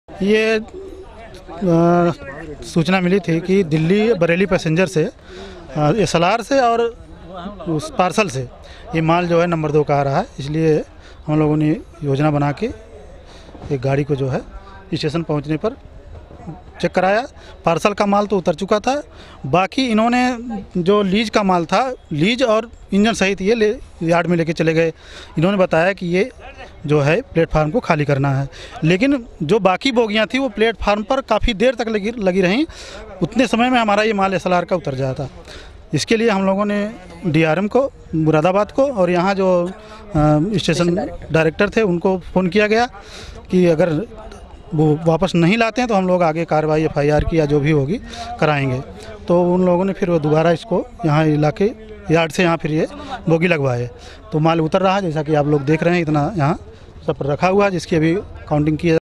कर विभाग का पड़ा छापा तो, इंजन और बोगी को लेकर भागा ड्राइवर
बरेली : रेलवे में टैक्स चोरी का मामला सामने आया है. अफसरों की मिलीभगत से सरकार को करोड़ों का चूना लगाया जा रहा है. इसका खुलासा बरेली जंक्शन पर हुआ. वाणिज्य कर विभाग की टीम ने अचानक छापा मारा, तो ड्राइवर इंजन समेत पार्सल बोगी को लेकर भाग गया.